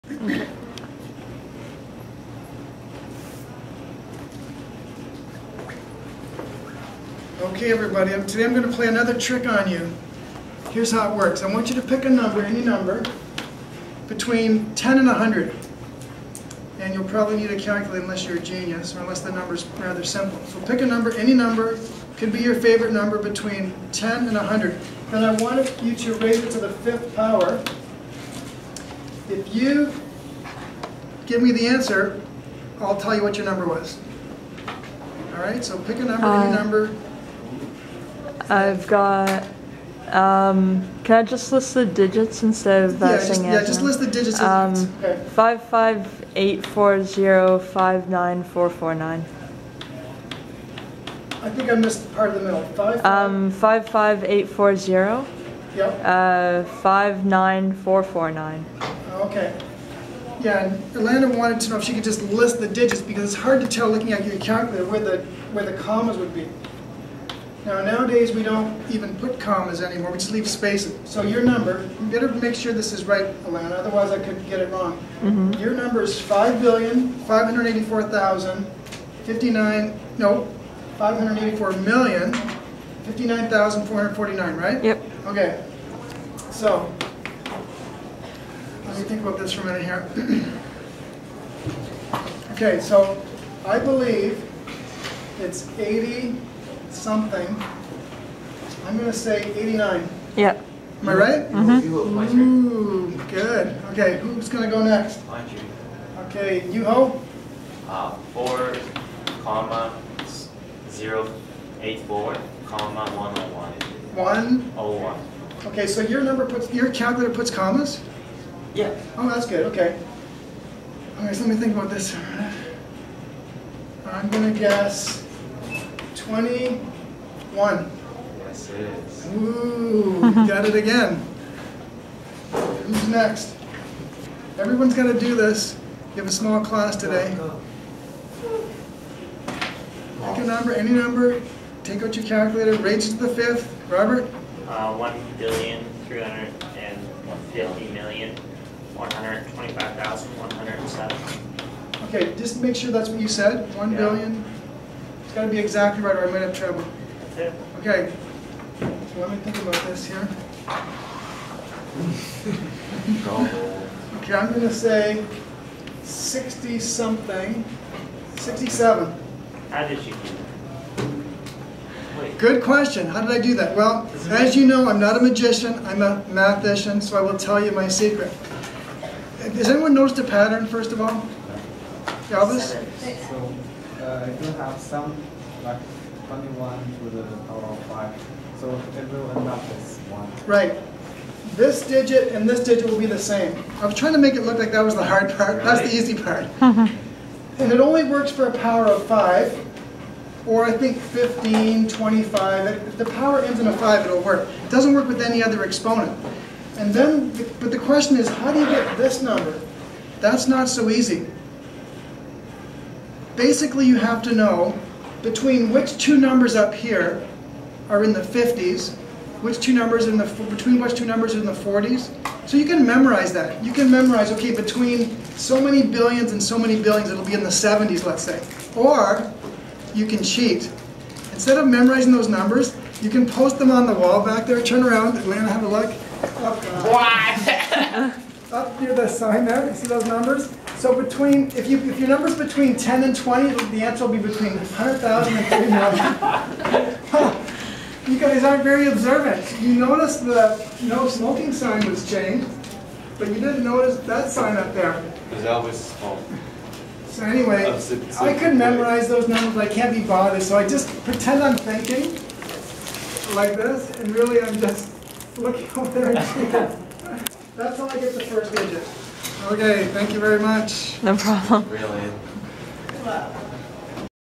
Taking the fifth root of a number.
Students choose any integer between 10 and 100, raise it to the fifth power, state the answer and Mr. J. Hush tells them their original number in a matter of seconds.